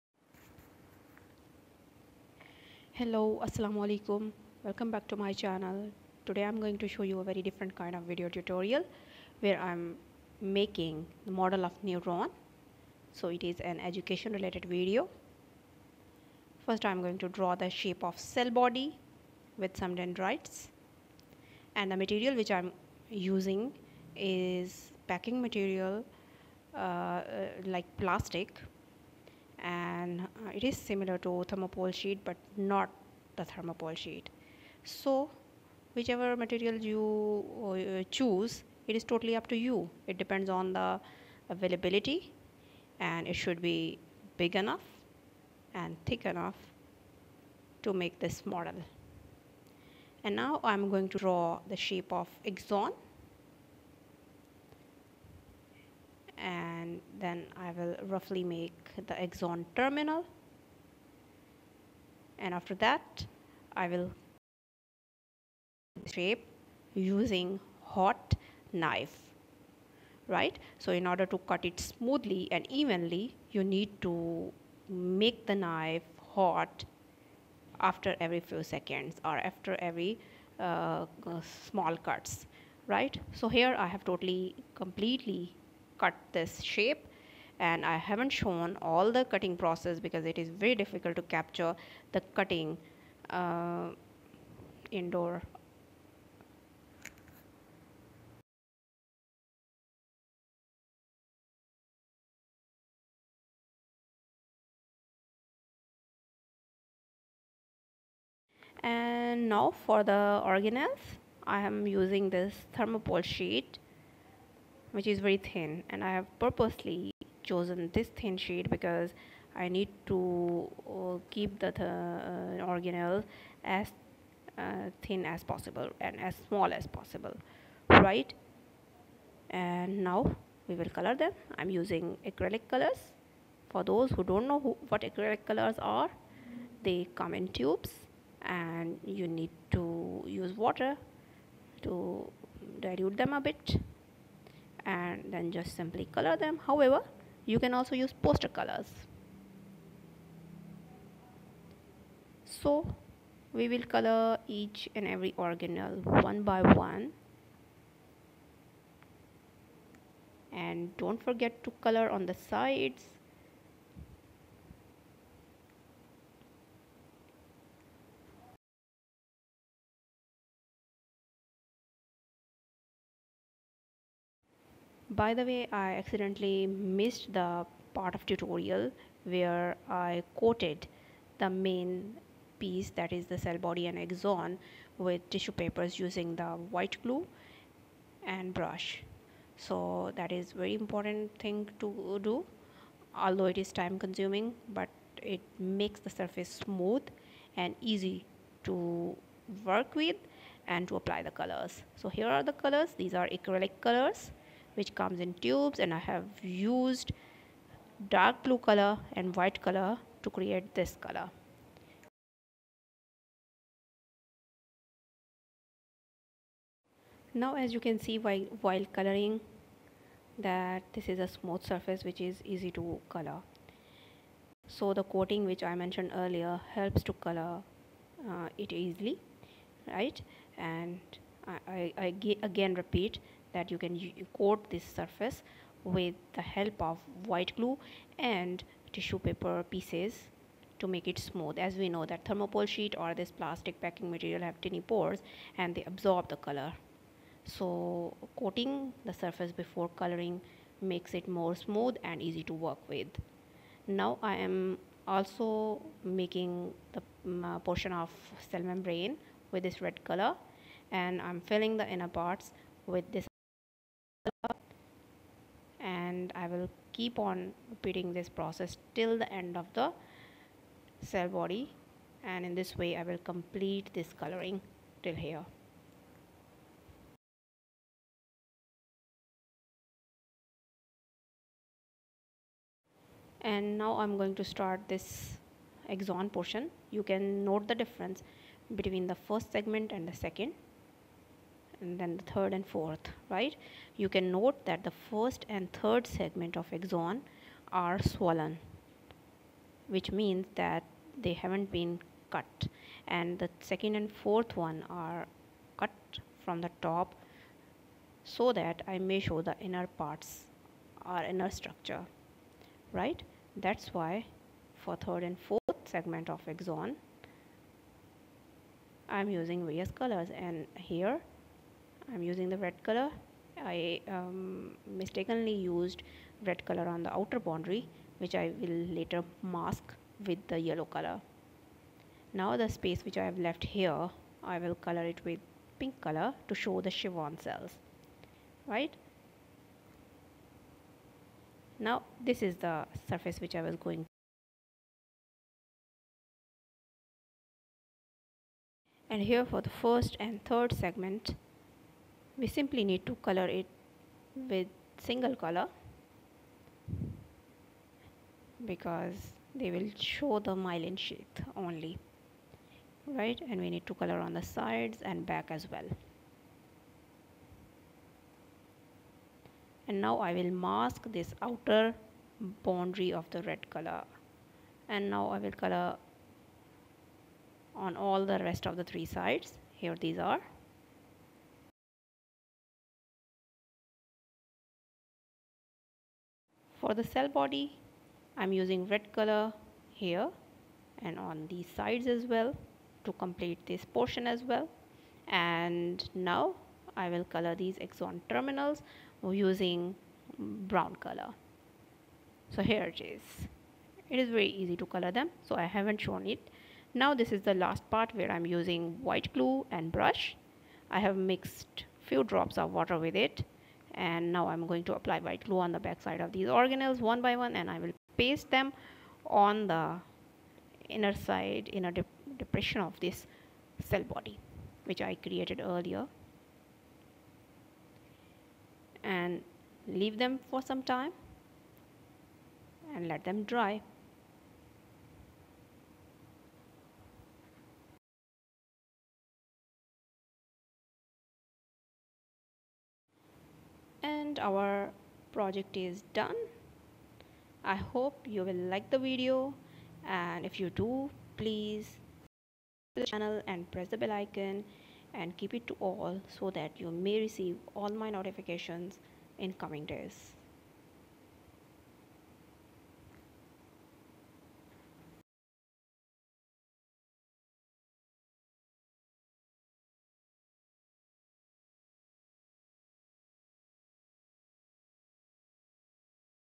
Neuron Model making/ Nerve cell biology 10th class model
Here i have given an educational video tutorial on how you can make a 3D model of Neuron for 10th class level.